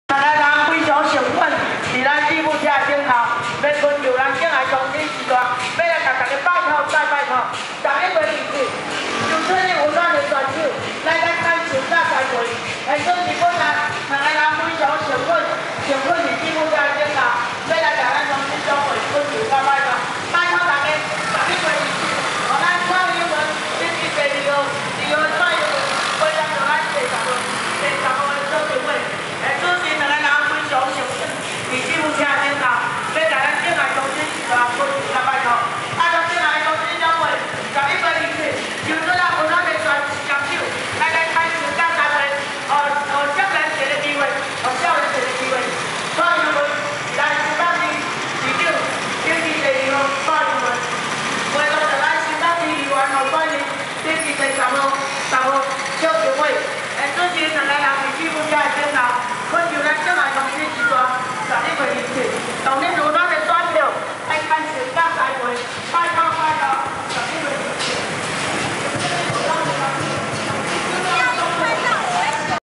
99.11.11晚上.新北市議員邱婷蔚和蔡英文三重市車隊掃街拜票03
99.11.11晚上.新北市議員邱婷蔚和蔡英文三重市車隊掃街拜票03
新北市議員(三重.蘆洲選區)邱婷蔚和小英市長
約略晚上7:30~8:00間